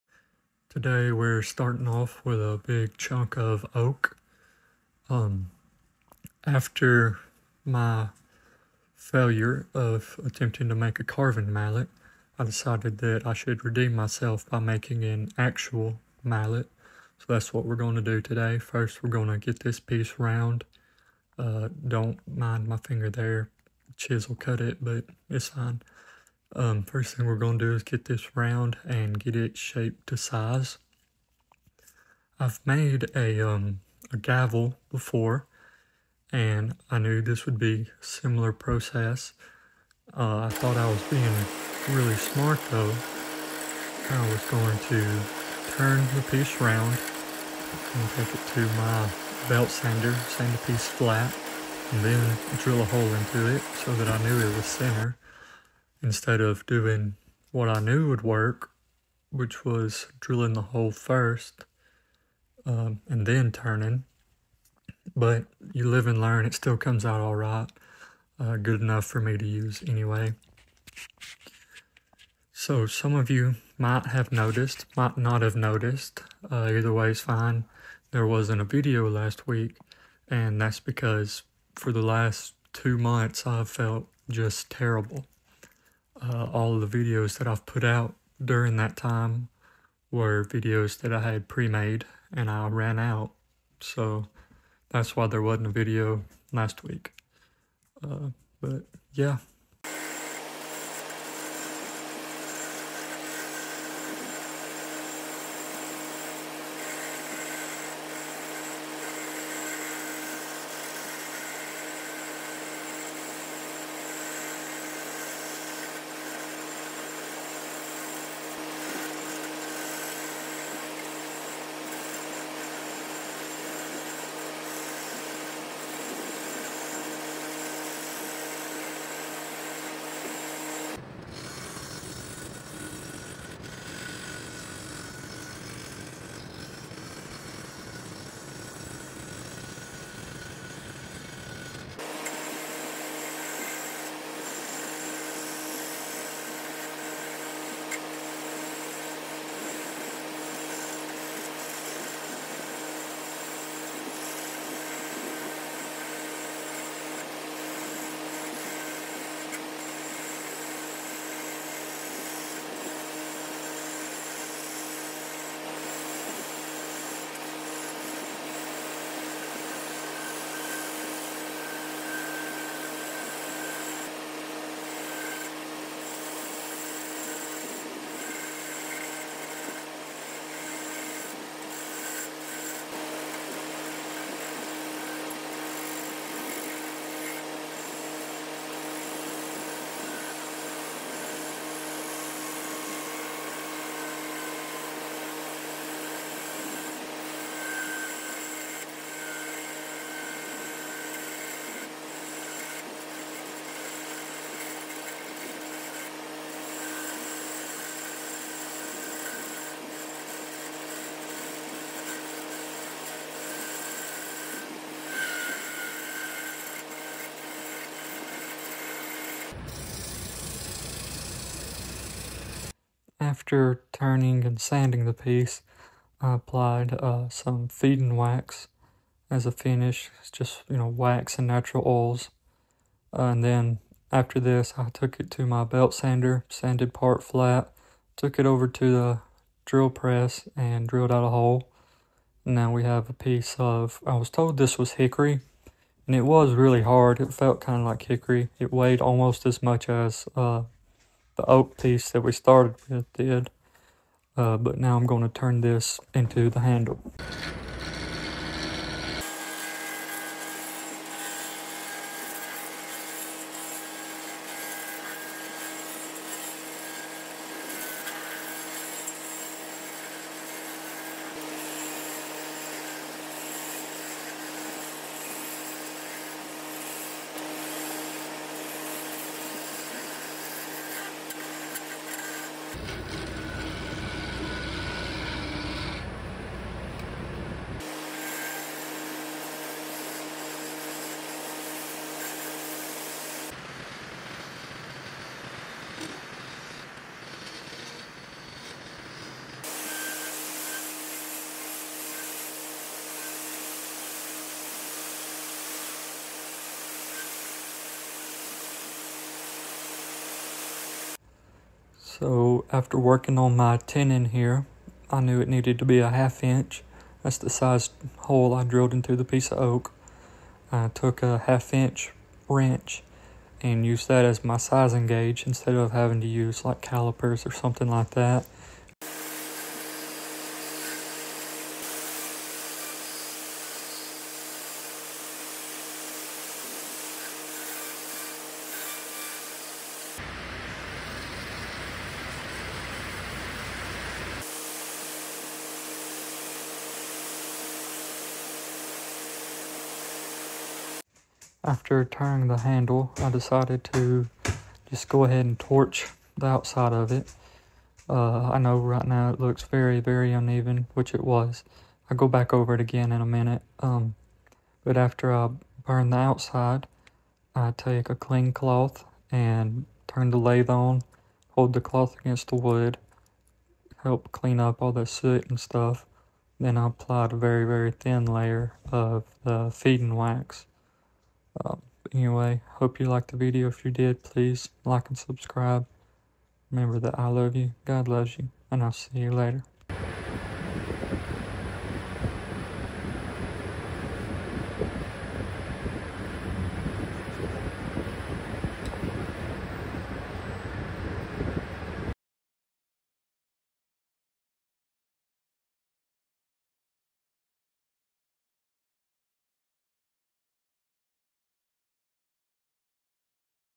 Making the Mallet
In this video I use oak and hickory to make a wooden mallet